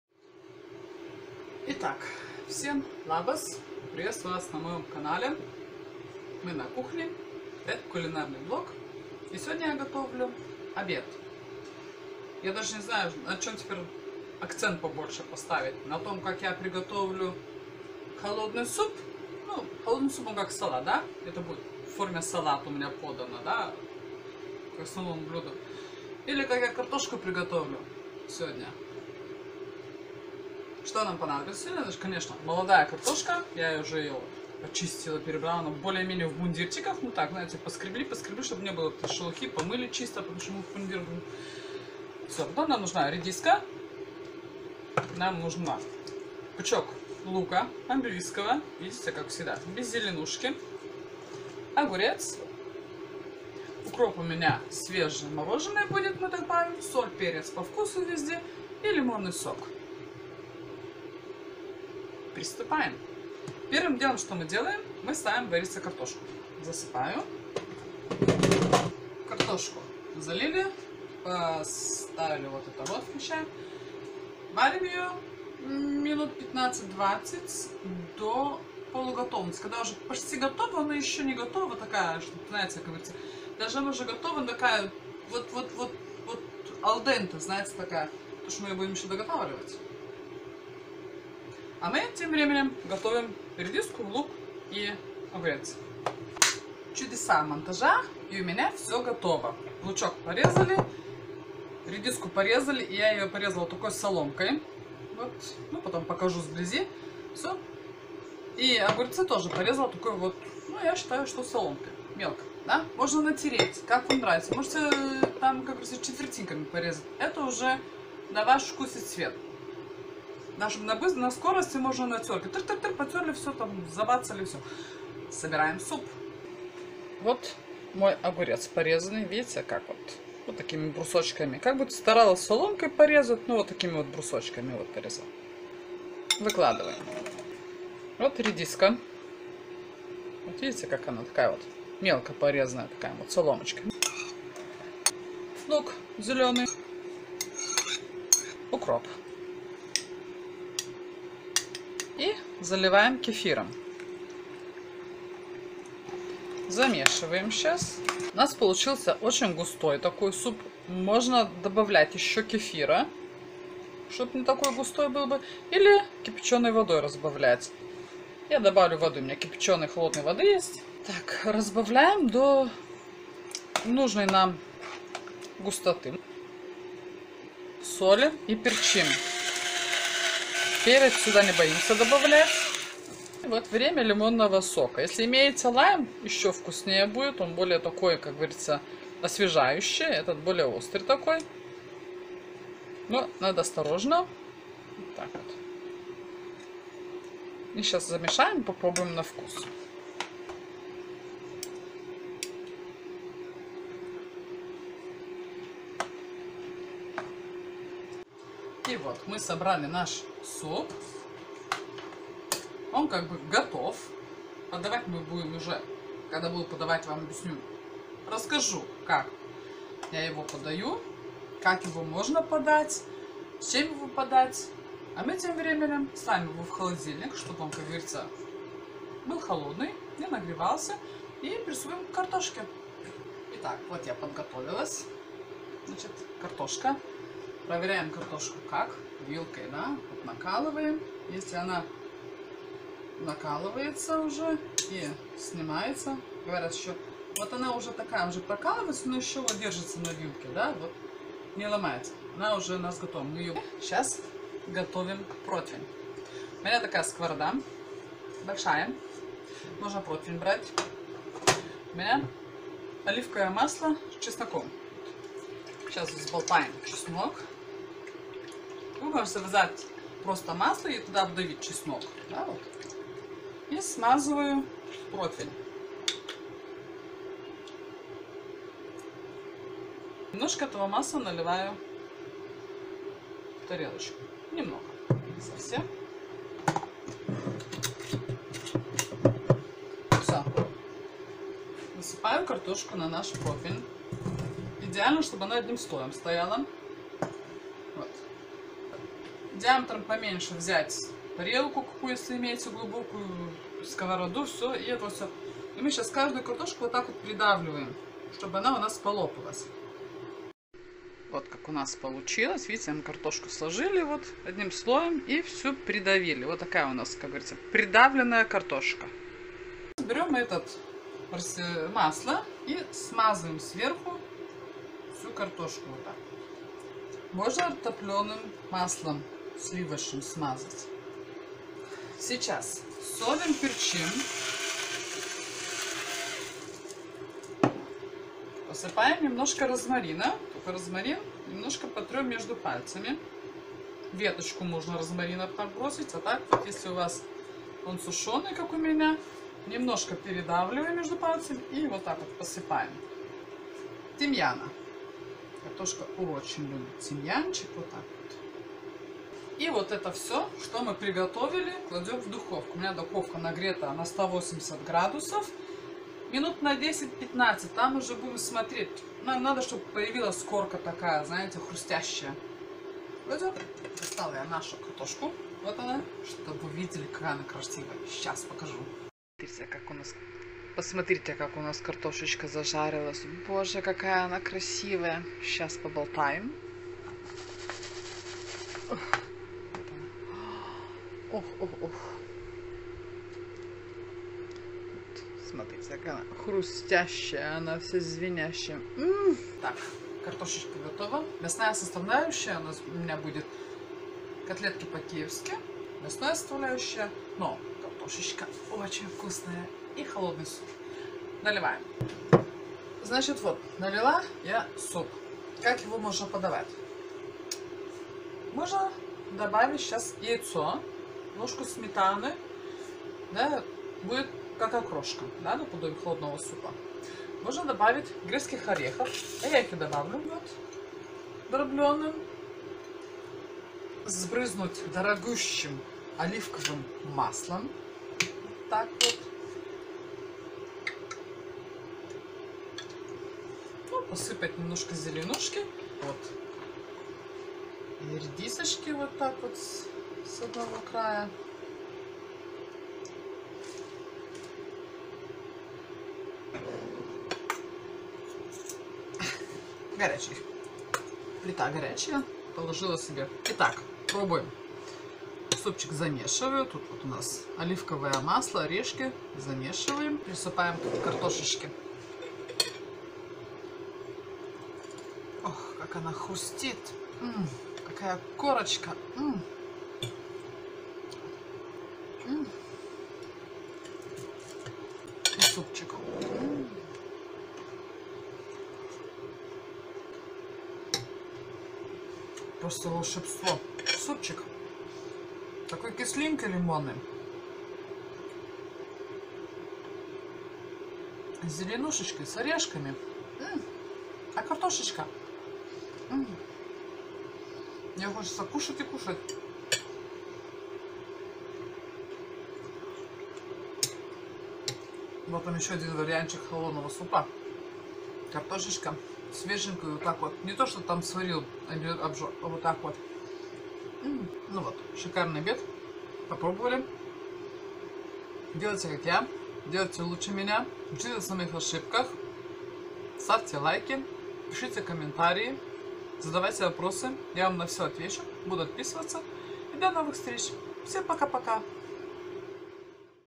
Хрустящий картофель и холодный суп!
Всем Labas, сегодня на обед готовим вкусный хрустящий картофель, в нем сплетается мякоть отварного картофеля и запеченная хрустящая корка. Это прекрасный гарнир к холодному супу. А холодник у нас сегодня на кефире с редиской. Это освежающий, вкусный и легко приготовляемый суп.
Приятного аппетита !